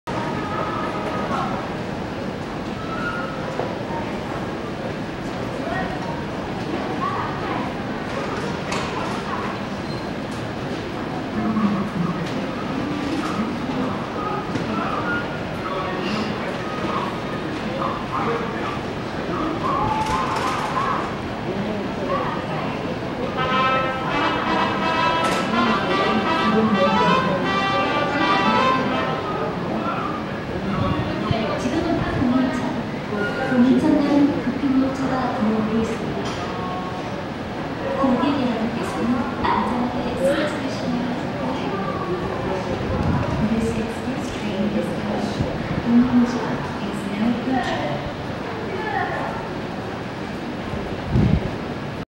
2017.09.20 경인선 1호선 부평역 동인천특급 도착 안내 전광판 표출 동영상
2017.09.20 경인선 1호선 부평역 동인천특급 도착 안내 전광판 표출 동영상